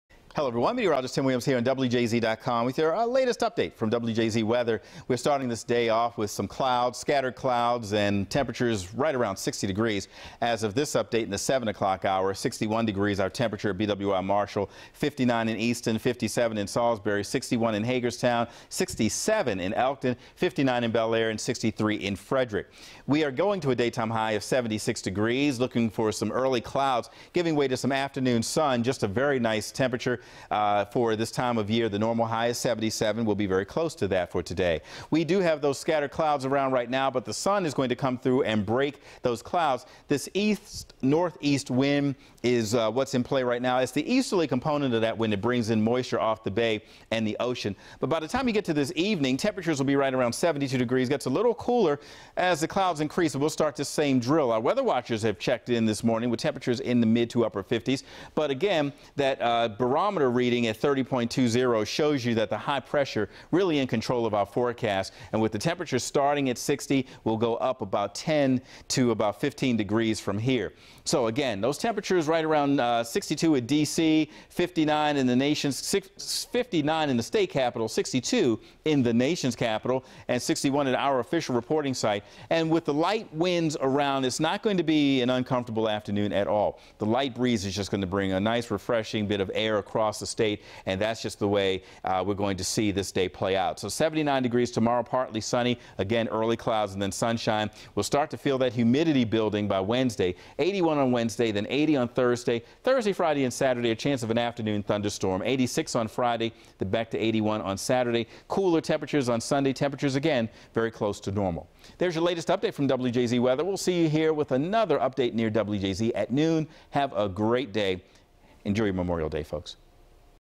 Monday Morning Weather With Meteorologist Tim Williams
Cloudy start, but the sun burns off most by the afternoon. High's in the mid 70's.